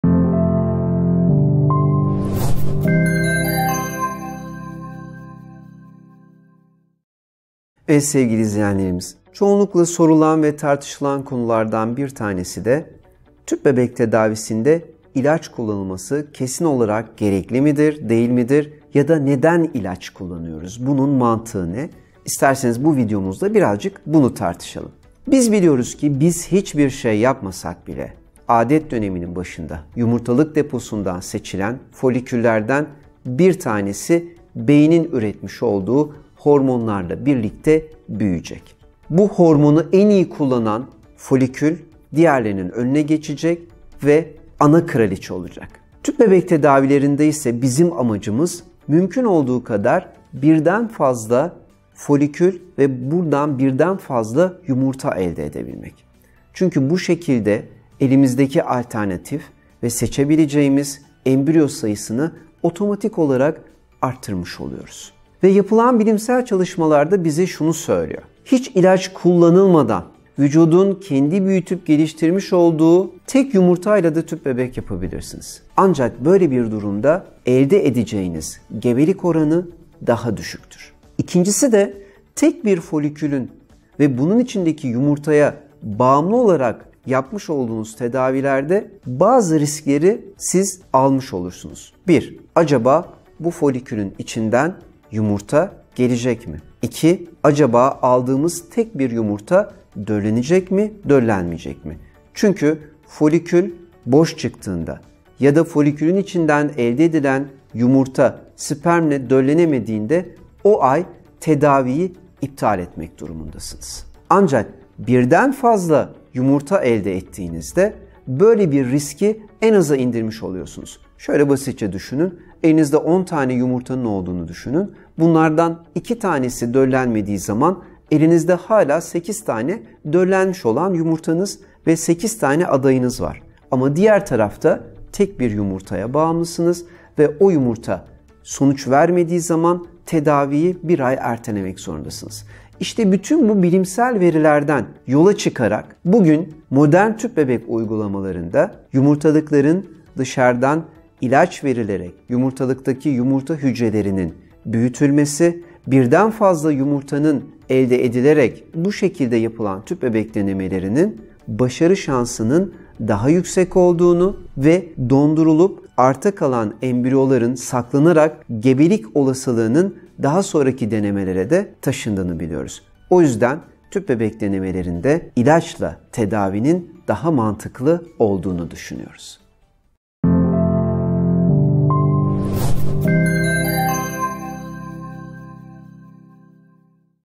TÜP BEBEK TEDAVİSİNDE İLAÇ KULLANMAK GEREKLİ MİDİR? #8
Op.Dr. Emre Karatekelioğlu sizler için anlatıyor, videoların devamı için kanalımıza üye olmayı unutmayın.